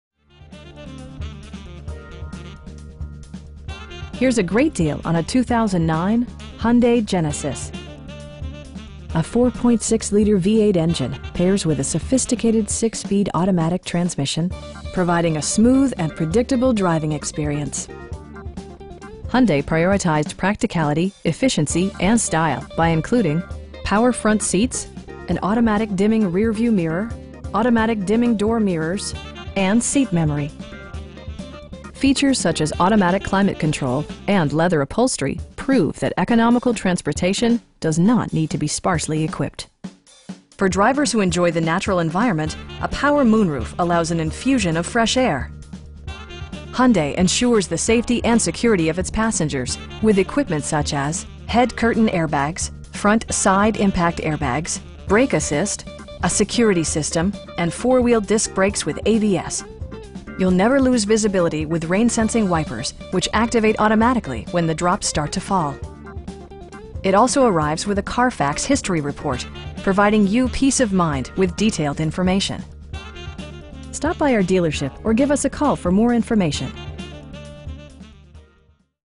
2009 Hyundai Genesis in Fort Walton Beach,, FL 32547
Quality Imports Toyota

Here's a great deal on a 2009 Hyundai Genesis. A 4.6 liter V-8 engine pairs with a sophisticated 6 speed automatic transmission, providing a smooth and predictable driving experience. Hyundai prioritized practicality, efficiency, and style by including: adjustable headrests in all seating position power front seats, an automatic dimming rear-view mirror, automatic dimming door mirrors, telescoping steering wheel, turn signal indicator mirrors, and seat memory. Features such as automatic climate control and leather upholstery prove that economical transportation does not need to be sparsely equipped. For drivers who enjoy the natural environment, a power moon roof allows an infusion of fresh air. Hyundai ensures the safety and security of its passengers with equipment such as: head curtain airbags, front SIDE impact airbags, traction control, brake assist, anti-whiplash front head restraints, a security system, and 4 wheel disc brakes with ABS. You'll never lose visibility with rain sensing wipers, which activate automatically when the drops start to fall. It also arrives with a Carfax history report, providing you peace of mind with detailed information. We offer competitive pricing, and the area's best shopping experience. Stop by our dealership or give us a call for more information.